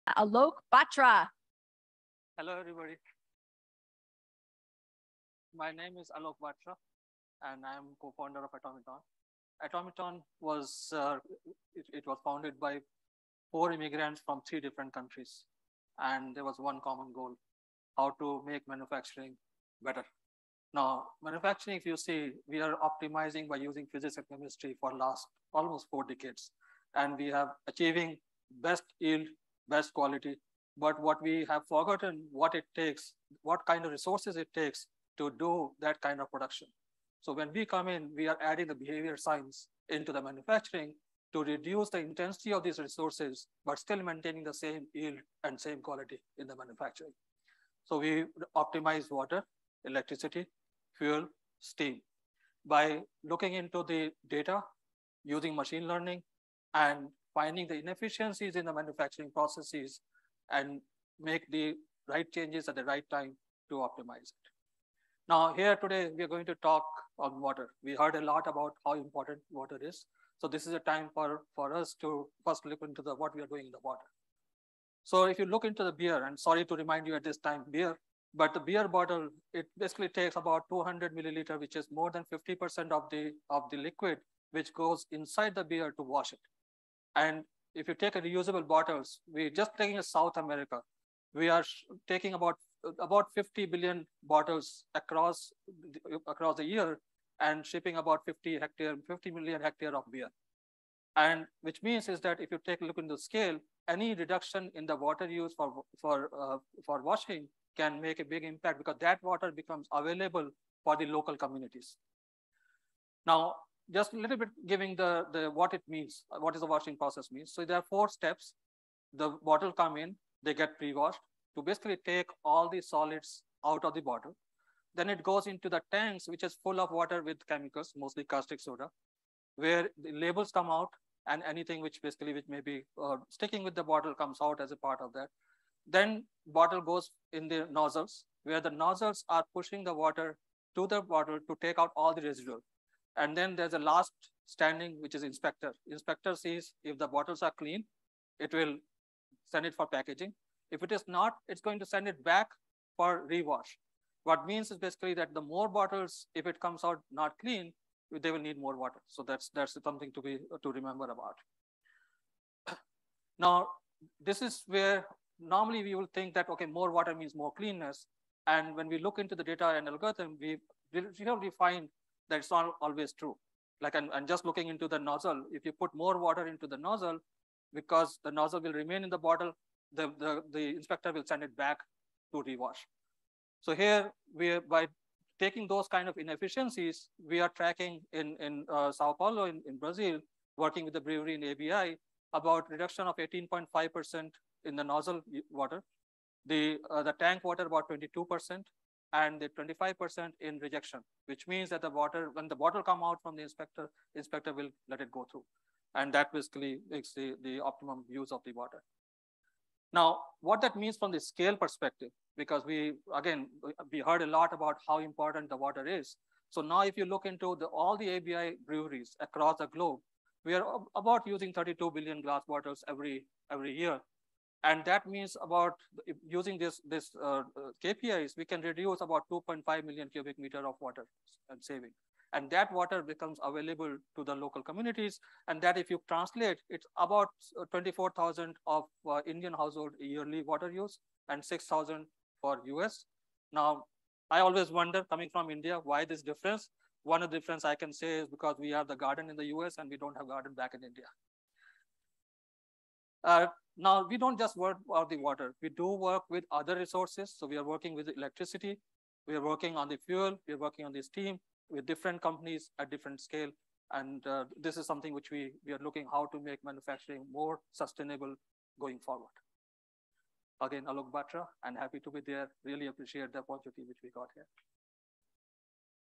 Atomiton + Demo Day
Atomiton is an artificial intelligence and machine learning software solution that identifies efficiencies in the manufacturing process.

Discover more about our startup Atomiton in this Demo Day pitch!